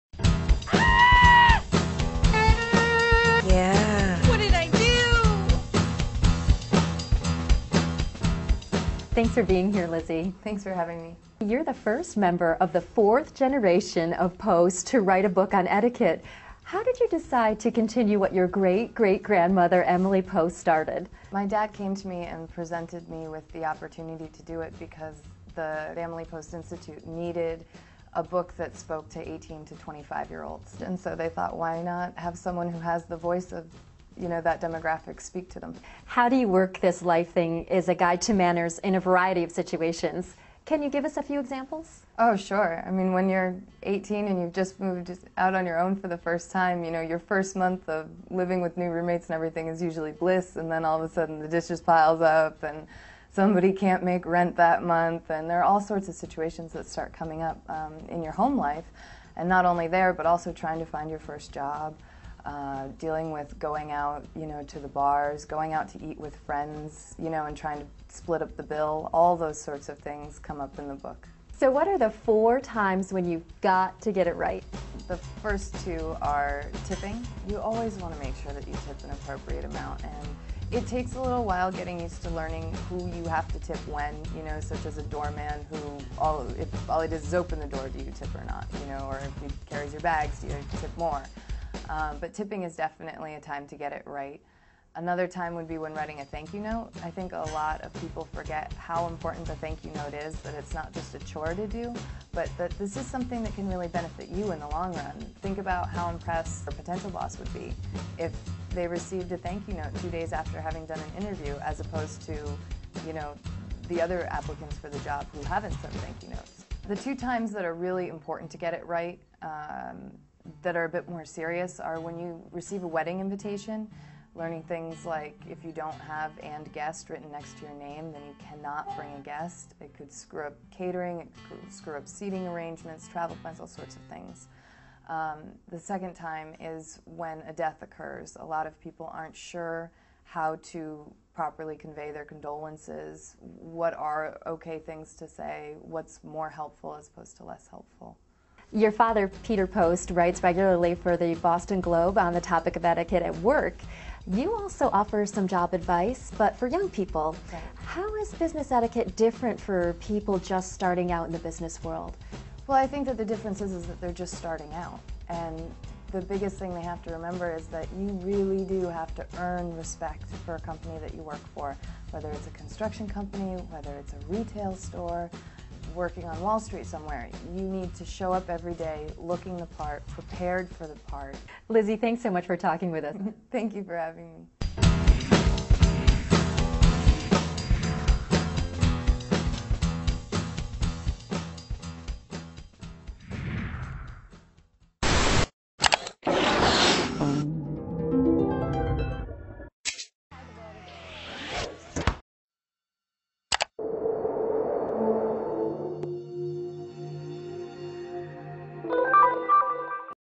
Lizzie Post - How Do You Work This Life Thing?
Lizzie Post, the great-great-granddaughter of Emily Post, joins her family's ranks of etiquette experts with her book, HOW DO YOU WORK THIS LIFE THING?  The book offers essential advice for the newly graduated and independent.